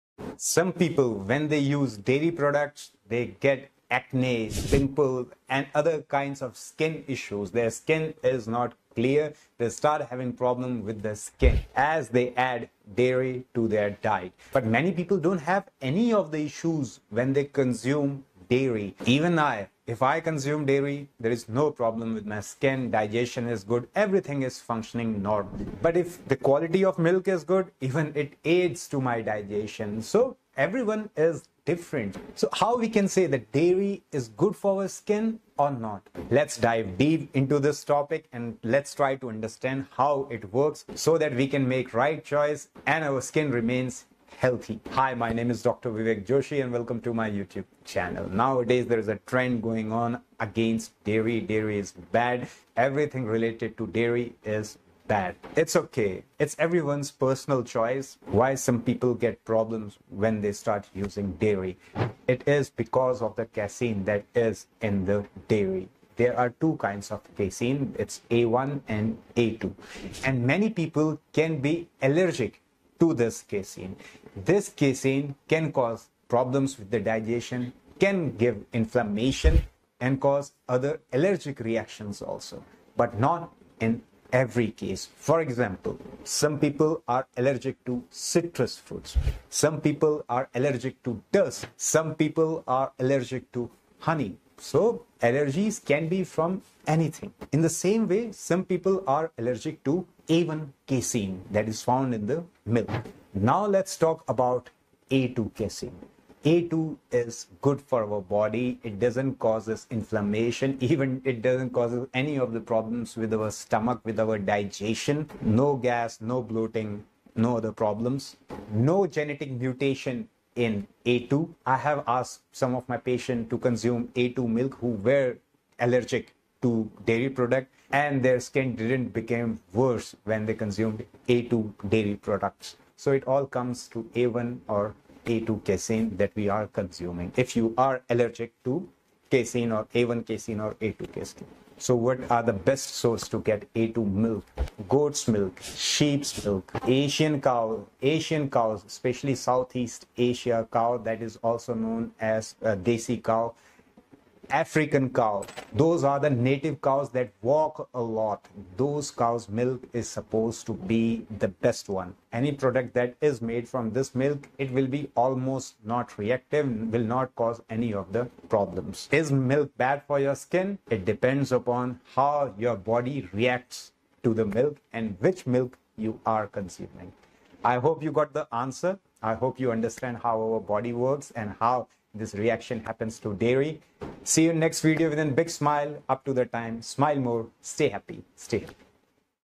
Milk Is Causing Pigmentation, Acne,Indigestion And Constipation - Dr. Vivek Joshi
Is Milk Bad For Skin ? Should You Avoid Milk And Dairy - Dr. Vivek Joshi
Pigmentation, Acne,Indigestion And Constipation